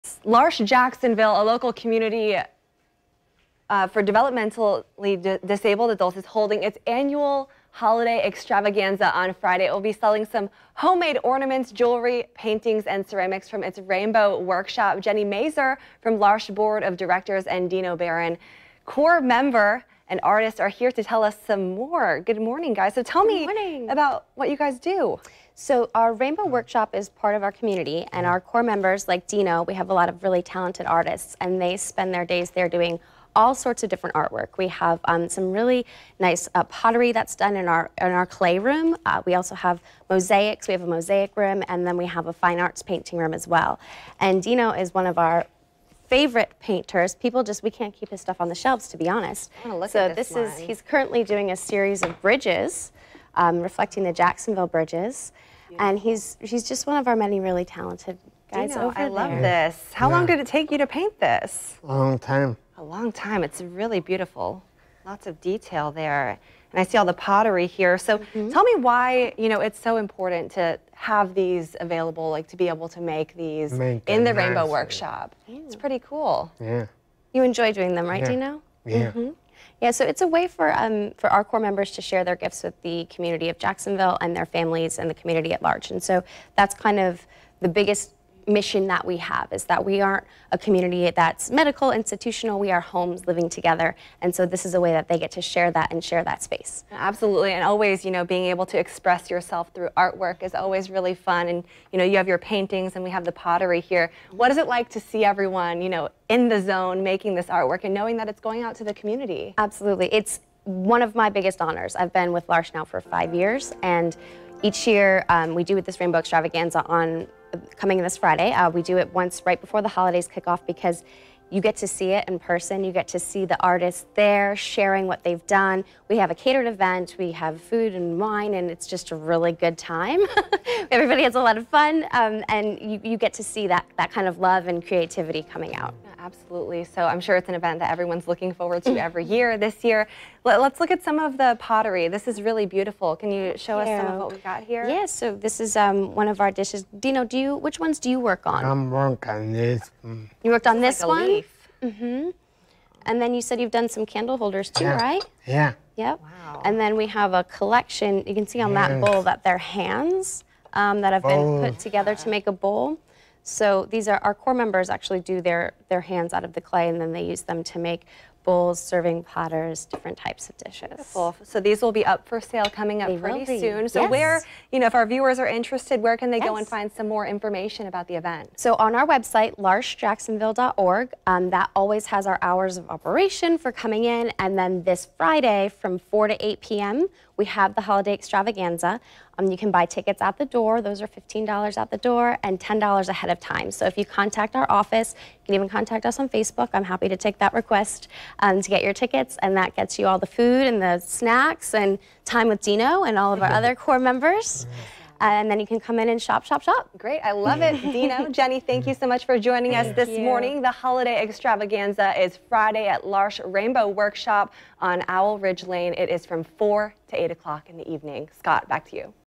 L'Arche Jacksonville holding its annual Holiday Extravaganza Friday
Jennie Mazur from the L'Arche board of directors, and Dino Baron, Core member and artist join us on the Morning Show with more.